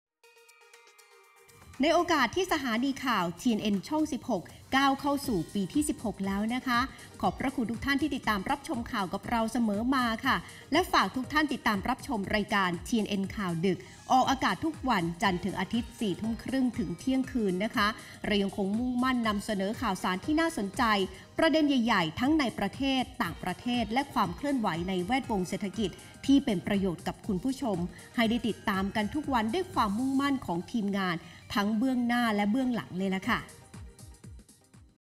คุณนิรัชญา ผู้ประกาศข่าวTNN กล่าว คำอวยพร ในโอกาสที่สถานี TNN ช่อง 16 ก้าวสู่ปีที่16
สถานีโทรทัศน์ TNN ช่อง 16 ก้าวสู่ปีที่ 16
คัดสรรทุกเนื้อหา และไม่หยุดพัฒนารูปแบบการนำเสนอ เพราะเราเชื่อว่าข่าวสารที่มีคุณค่า เต็มเปี่ยมด้วยสาระประโยชน์ จะช่วยเพิ่มคุณภาพชีวิตและเป็นฟันเฟืองสำคัญในการผลักดันประเทศในทางที่ดีต่อไป